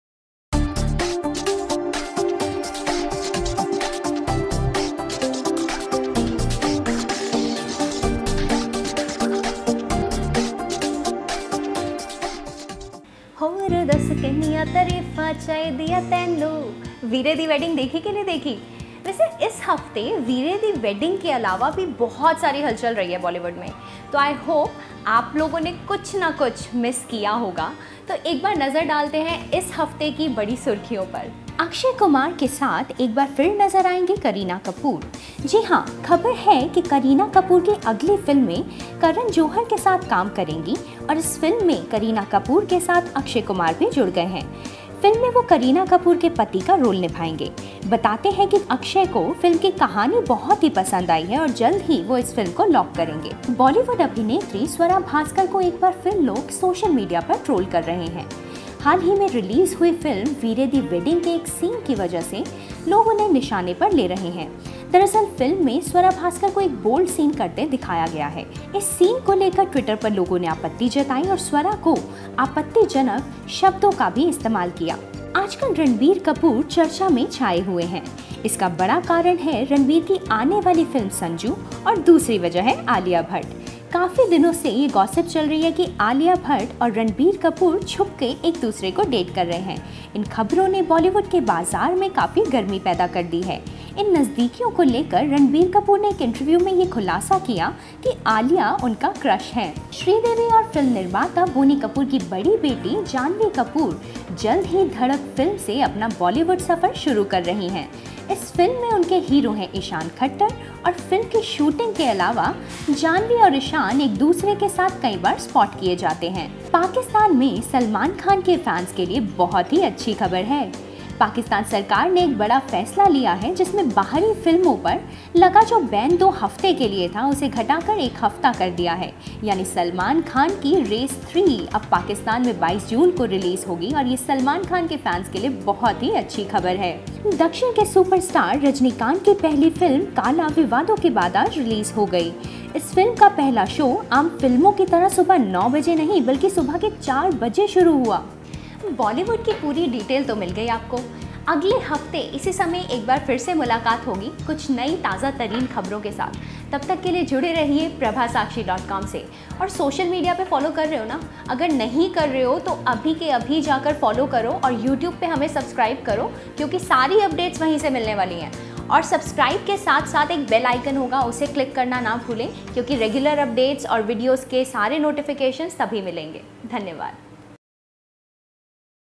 weekly big Bollywood news, बॉलीवुड गलियारों सुपरहिट गॉसिप
इस सप्ताह का हुआ बॉलीवुड गलियारों में देखिए इस वीडियों में बड़ी खबरें

1 अक्षय कुमार के साथ एक बार फिर नजर आएंगी करीना कपूर- ख़बर है कि करीना कपूर की अगली फिल्म में करण जौहर के साथ काम करेंगी और इस फिल्म में करीना कपूर के साथ अक्षय कुमार भी जुड़ गए हैं।

2 बॉलीवुड अभिनेत्री स्वरा भास्कर को एक बार फिर लोग सोशल मीडिया पर ट्रोल कर रहे हैं. हाल में रिलीज़ हुई फ़िल्म 'वीरे दी वेडिंग' के एक सीन की वजह से लोग उन्हें निशाने पर ले रहे हैं.

3 काफी दिनों से ये गॉशिप चल रही है कि  रणबीर चुपचुप कर आलिया भट्ट को डेट कर रहै हैं। इन खबरों ने बॉलीवुड के बाजार में काफी गर्मी पैदा कर दी है। इन नजदीकियों को लेकर रणबीर कपूर ने एक इंटरव्यू में ये खुलासा किया कि आलिया उनका क्रश हैं।

 4  ‘फ़िल्म ‘धड़क’ की शूटिंग के अलावा भी जाह्नवी और ईशान एक दूसरे के साथ कई बार स्पॉट किये जाते रहे हैं।

5 पाकिस्तान में सलमान खान के फैंस के लिए अच्छी खबर... पाकिस्तान सरकार ने एक बड़ा फैसला लिया हैं सलमान खान की रेस 3 अब पाकिस्तान में 22 जून को रिलीज होगी।

 6 दक्षिण के सुपरस्टार रजनीकांत की फिल्म ‘काला’ विवादों के बाद आज रिलीज हुई। इस फिल्म का पहला शो आम फिल्मों की तरह सुबह 9 बजे नहीं,बल्कि सुबह 4 बजे शुरू हुआ।